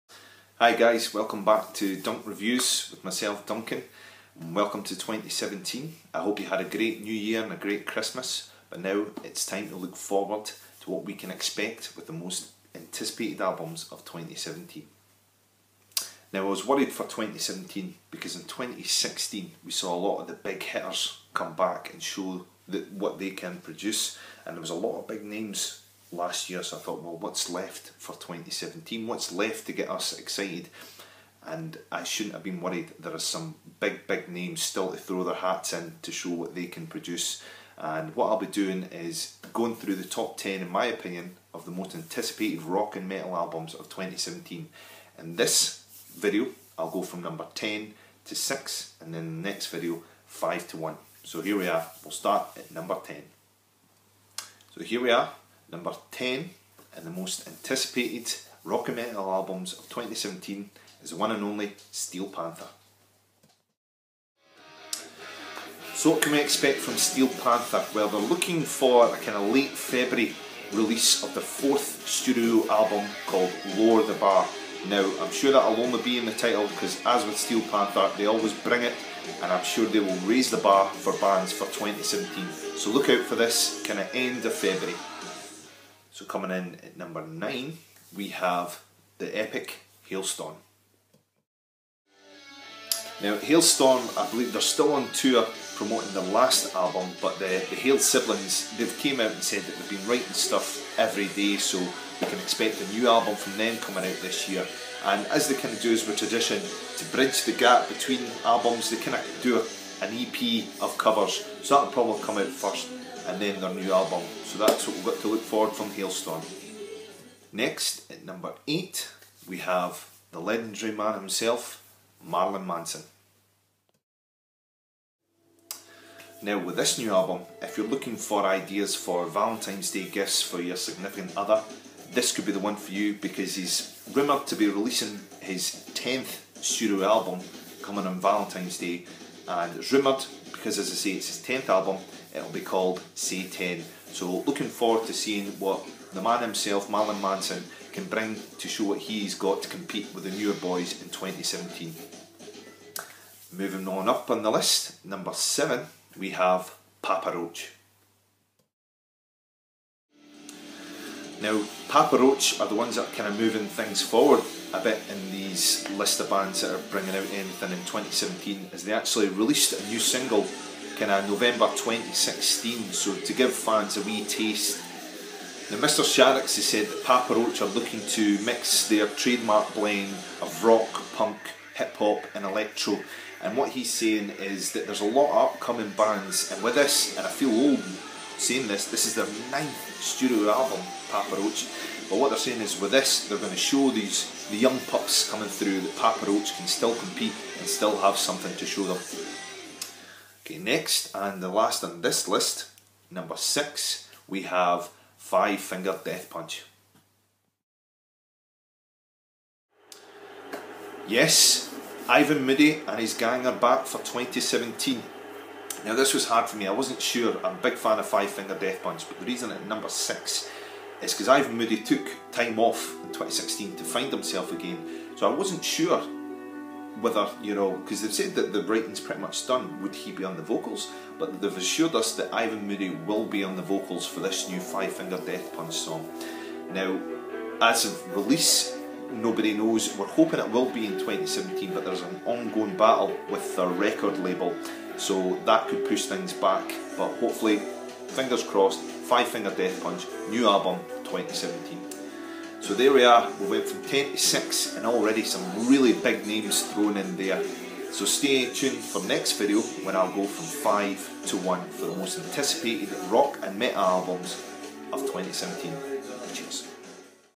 Most anticipated Rock & metal album releases 2017
A run down from number 10 to number 6 of the top 10 Rock and albums being released in 2017! Numbers 5 to number 1 will be in the next video

I do not own or claim to own the rights of any of these clips they are used for review purposes only.